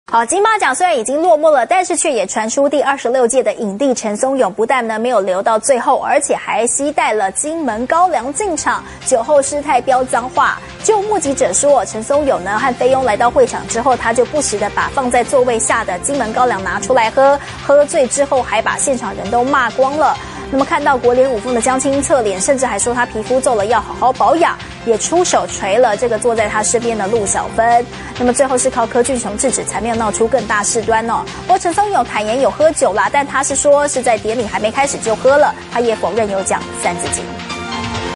中天新聞》陳松勇金馬典禮喝茫 被爆酒後飆髒話
中天新聞》金馬獎圓滿落幕！但是卻傳出第26屆影帝陳松勇帶了金門高粱進場，還酒後失態飆髒話，目擊者說陳松勇與菲傭到會場後，他就不時把放在座位下的金門高粱拿來喝，喝醉後把在場的人罵光光，看到國聯五鳳的江青側臉，說她皮膚皺了要她好好保養，還出手猛捶坐在他身邊的陸小芬。最後是靠柯俊雄制止，才沒鬧出更大事端。